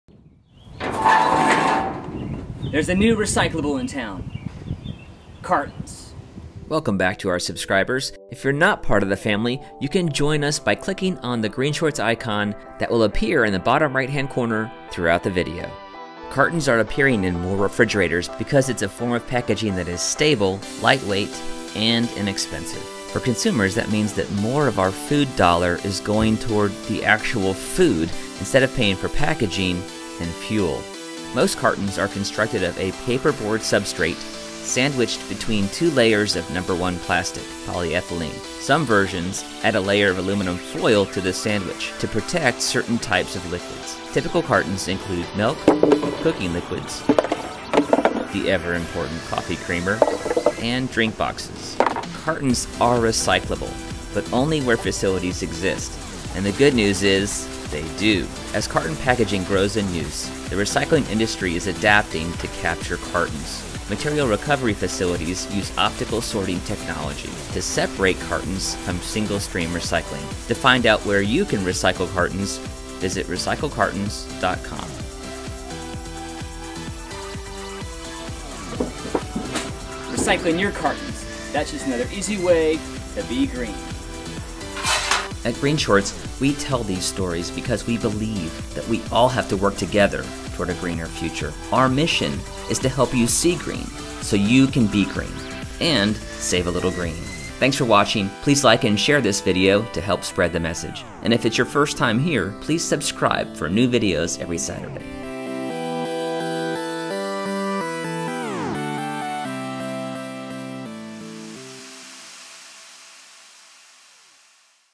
How To Recycle Cartons
Cartons are recyclable, but only where recycling facilities exist with the right type of sorting technology, usually optical sorters. The good news is that because carton packaging is growing in use, the number of MRFs (Material Recovery Facilities) that accept them is growing.

Even if your curbside recycling program doesn't yet accept cartons, you may be able to take your to a local recycling center. Find out if you have a recycler that takes cartons by visiting recyclecartons.com.